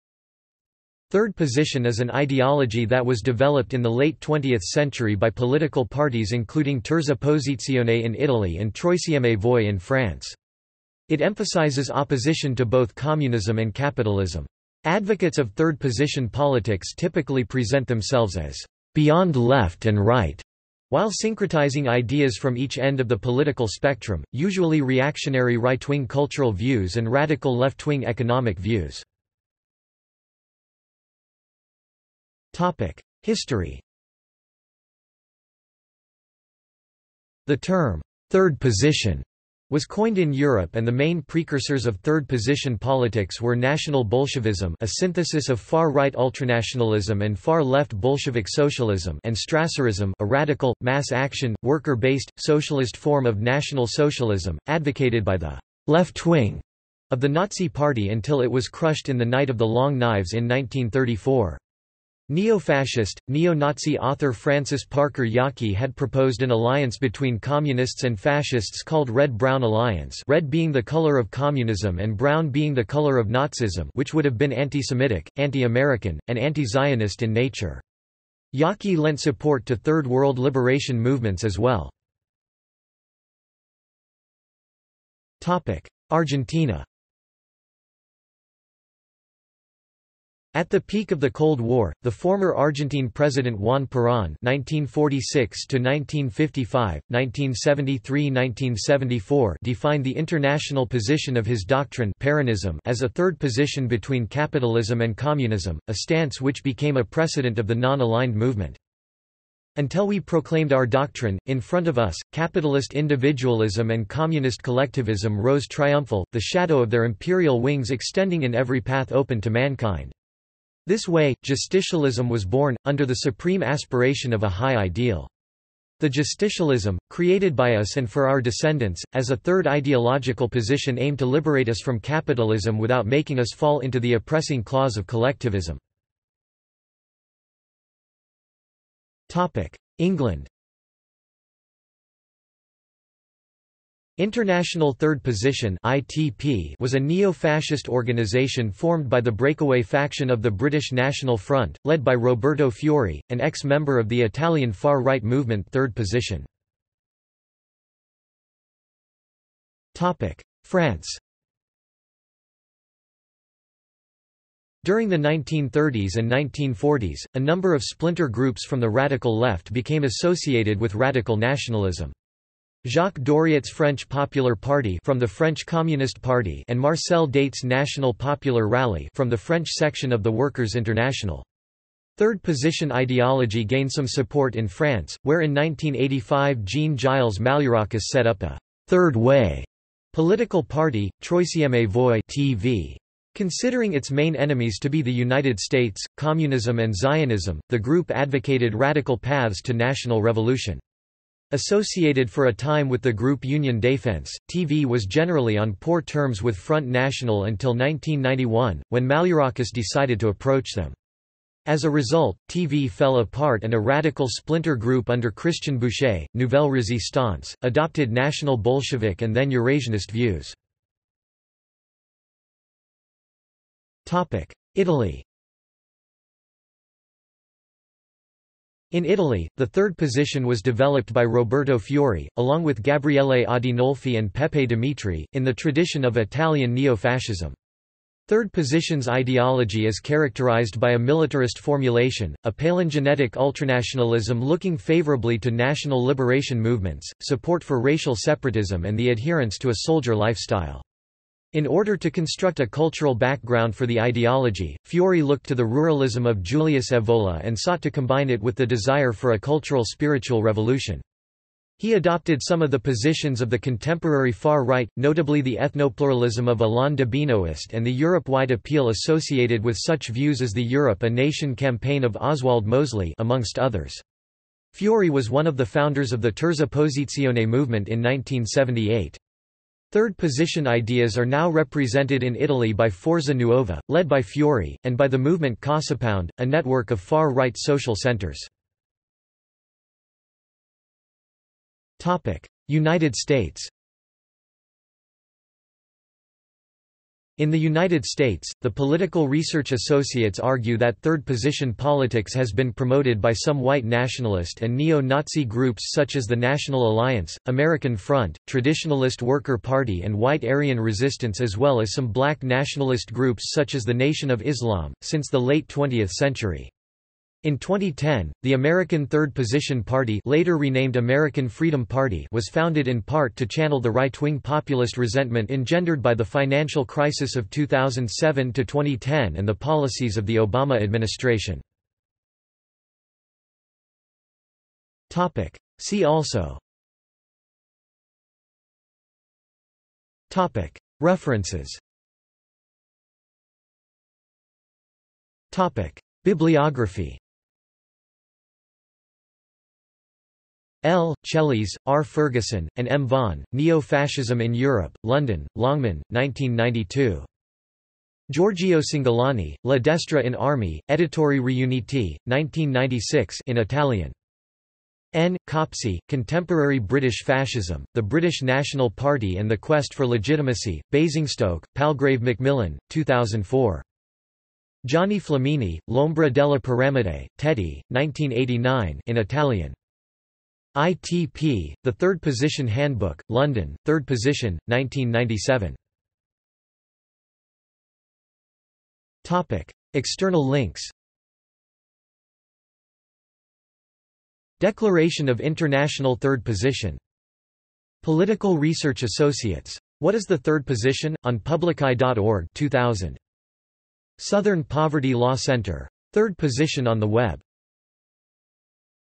Third Position | Wikipedia audio article
This is an audio version of the Wikipedia Article:
Third Position

- Socrates

SUMMARY
Third Position is an ideology that was developed in the late 20th century by political parties including Terza Posizione in Italy and Troisième Voie in France. It emphasizes opposition to both communism and capitalism. Advocates of Third Position politics typically present themselves as "beyond left and right" while syncretizing ideas from each end of the political spectrum, usually reactionary right-wing cultural views and radical left-wing economic views.